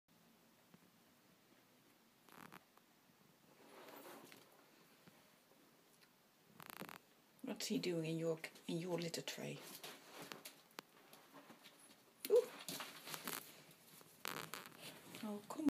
Bors the half-tailed ferret on litter tray
Bors the half-tailed ferret takes the piss out of cats by squatting in their litter tray.